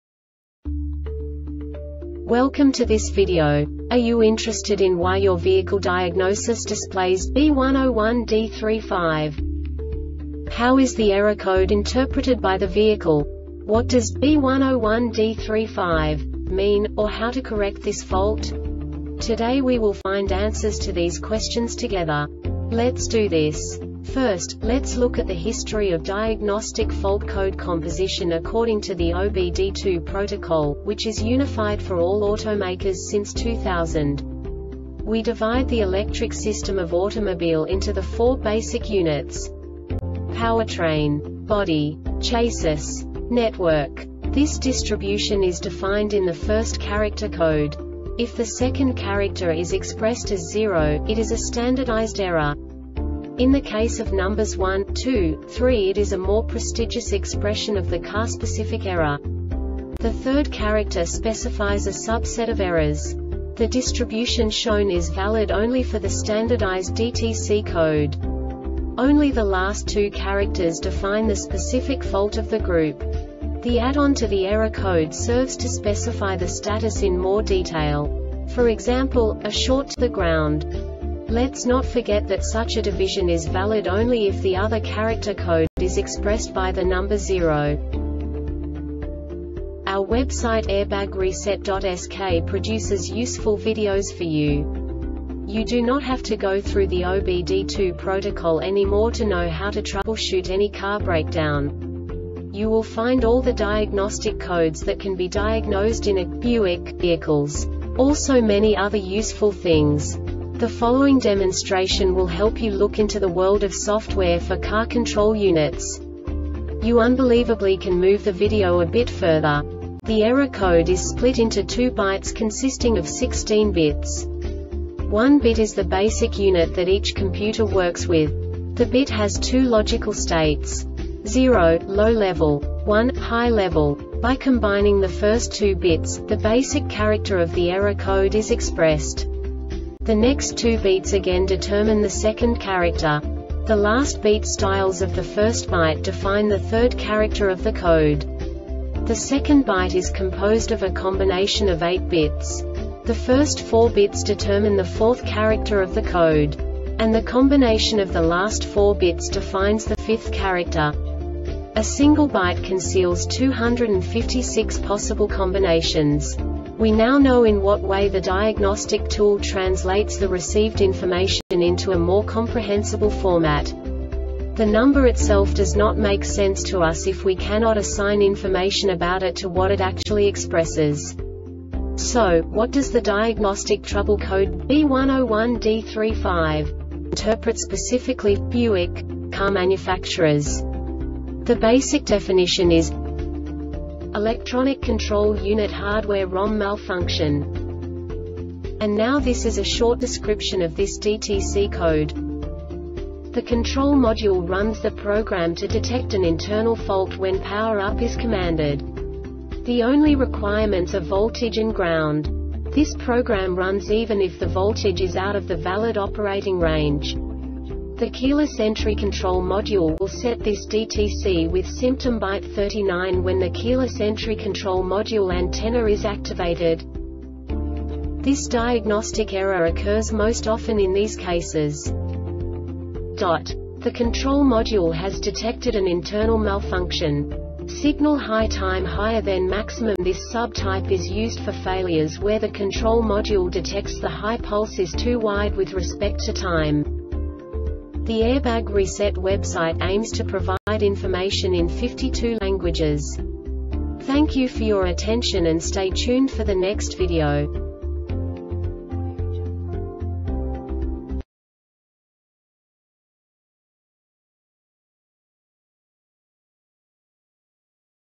DTC Buick B101D-35 Short Explanation
Contents:
0:21 Basic DTC analysis according to OBD2 protocol standard.
1:48 Insight into programming
2:58 A diagnostically understandable description of the Diagnostic Trouble Code
3:05 Basic error definition

Make:
Buick
Code:
B101D 35
Definition:
No Body Bus Message
Cause:
Failure Type:
Signal High Time  higher then Maximum
This sub type is used for failures where the control module detects the high pulse is too wide with respect to time.